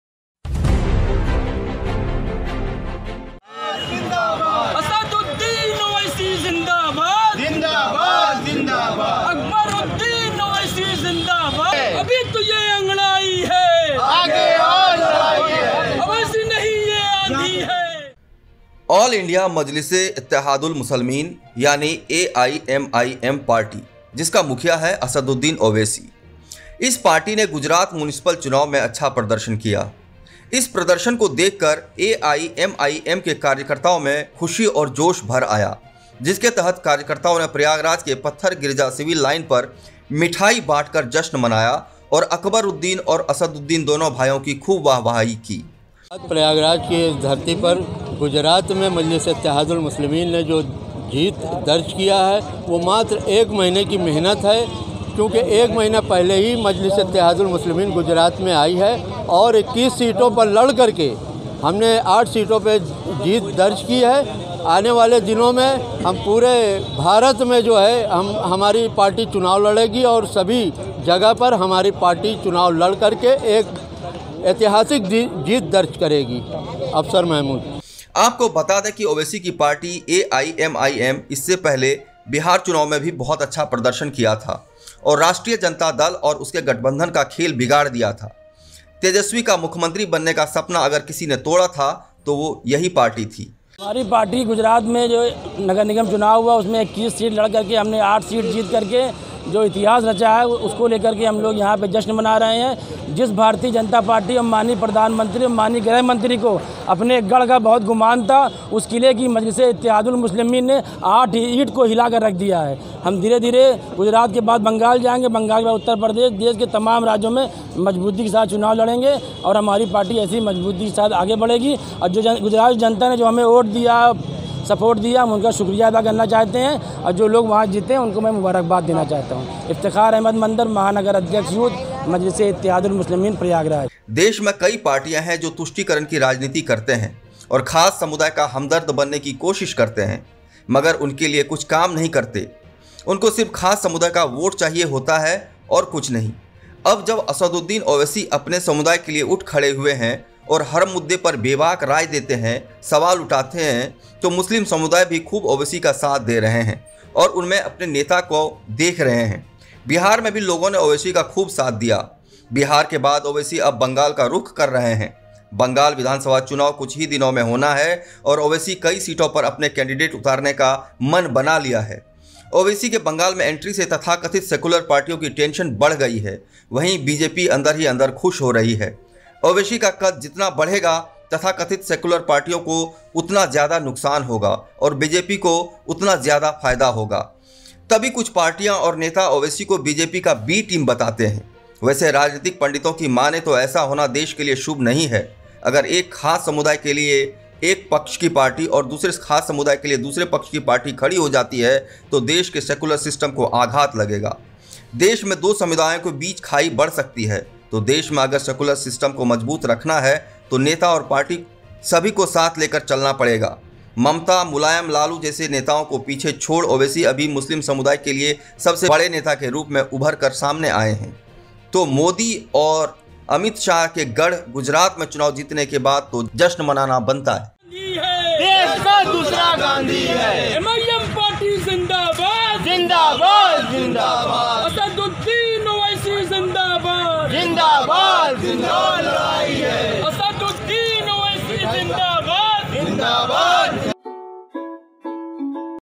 ओवैसी के बढ़ते कद से कई राजनीतिक दलों में खलबली । Owaisi In Gujrat
ऑल इंडिया मजलिस ए इत्तेहादुल मुस्लिमीन AIMIM की गुजरात म्युनिसिपल चुनाव में सफलता पर मिठाई वितरण  पत्थर गिरजा सिविल लाइन पर मिठाई वितरण  के साथ जश्न मनाया जाएगा l